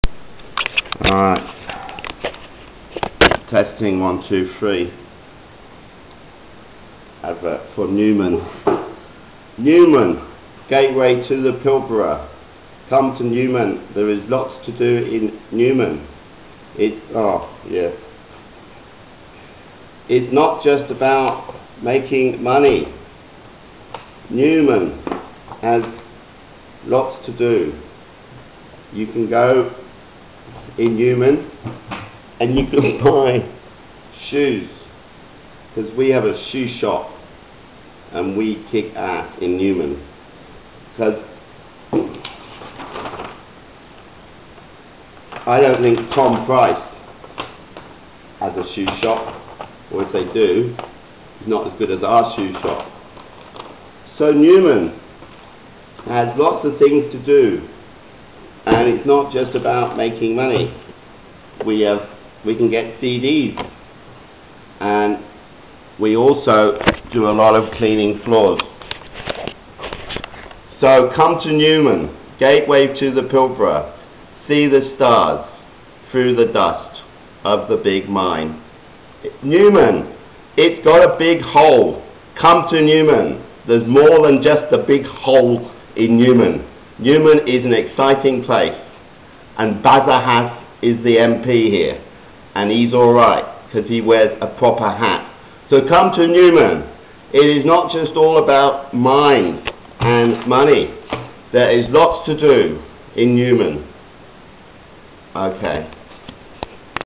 Gateway to the Pilbara, Newman its more than a Big Hole
Advert for Newman Gateway to the Pilbara.  See the stars thru the dust. Its not just about making money or our Big Hole. We have a shoe shop in Newman and we mop floors.  There is lots to do in Newman, Western Australia's Cultural Capital.